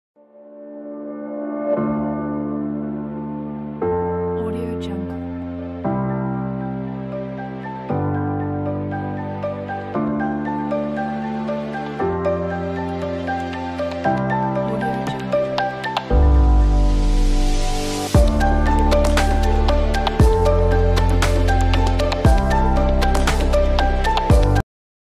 Topographic Parallax Slideshow After Effects Template
Features:
21 Placeholders for Photos or Videos
Modular Structure
Color Control
Easy to edit, video tutorial included
After Effects CS5.5 and above
Universal Expressions